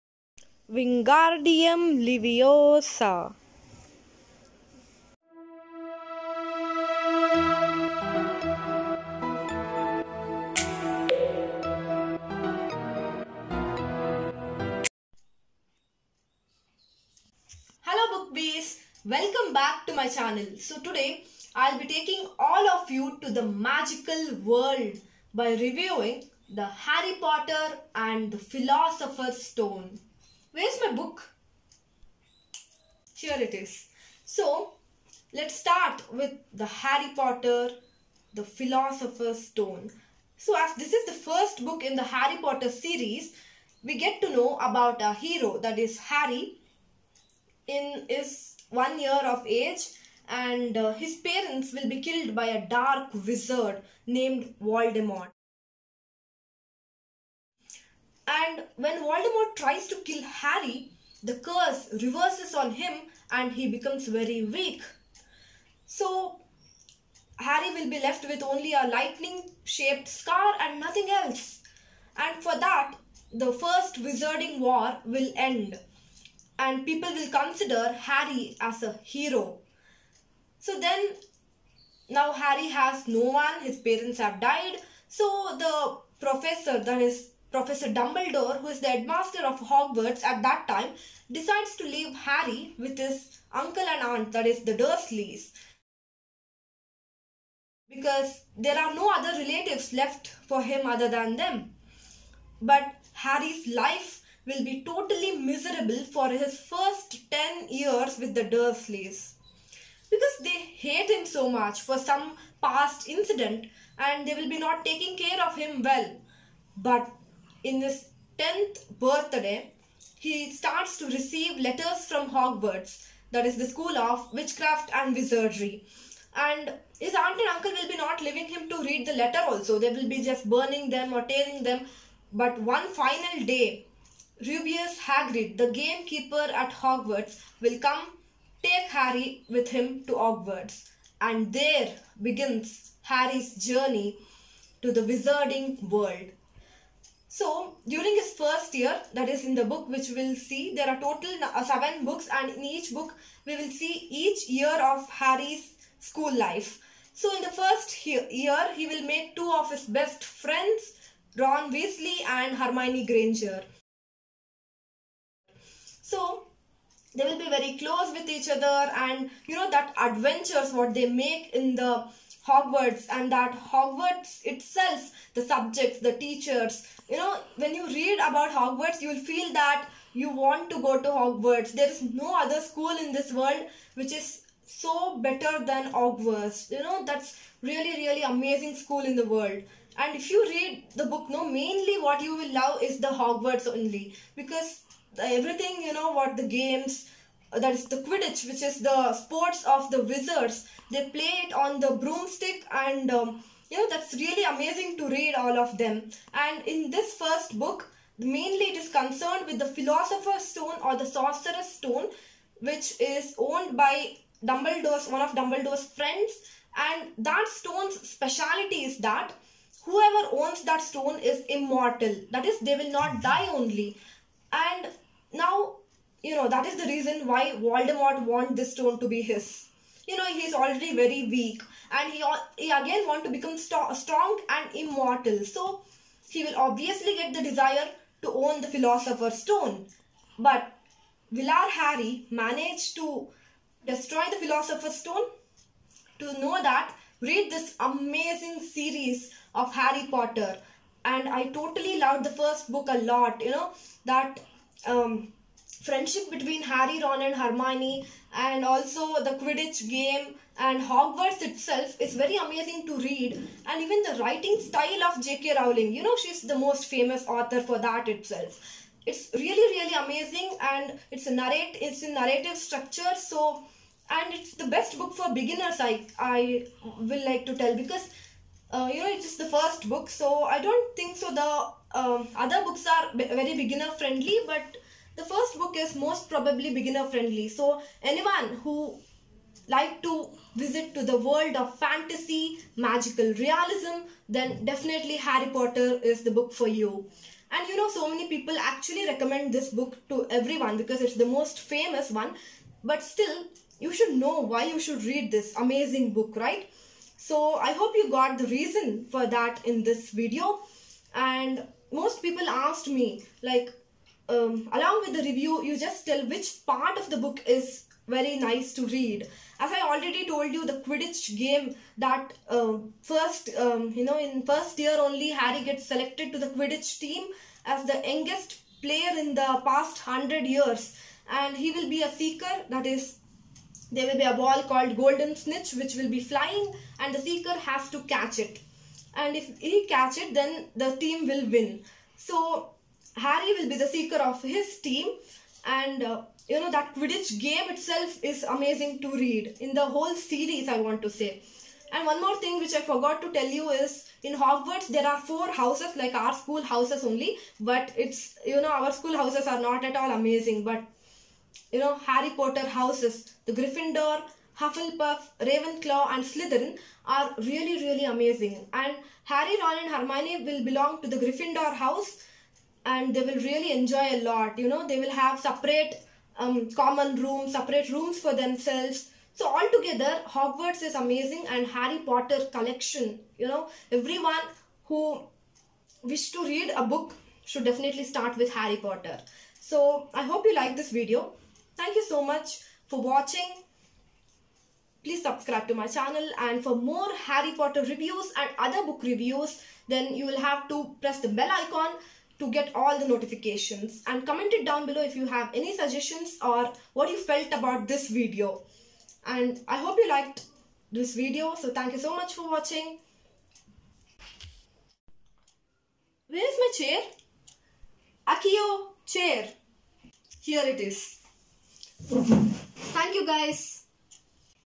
Harry Potter review | With magic and spells!!
Hello guys after a long time!!
Hope you will like the review. I was planning to review this book for a long time but time did not permit till now. So here it is Harry Potter and the Philosopher's Stone. With SOME MAGIC. I really enjoyed filming this video hope you too enjoyed watching it.